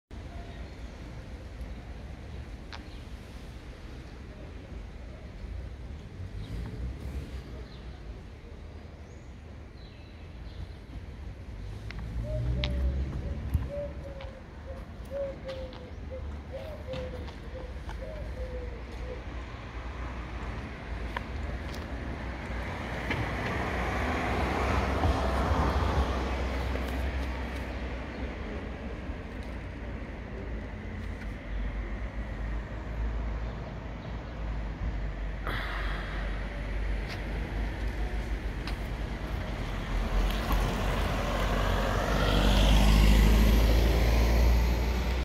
Velo de Ville trekking AEB-900.
Cuadro abierto aluminio, motor bosch CX performance, 🔋 400W, cambio XT 12v, suspensión delantera, rueda 700,kit completo de guardabarros, portabultos, luces, timbre.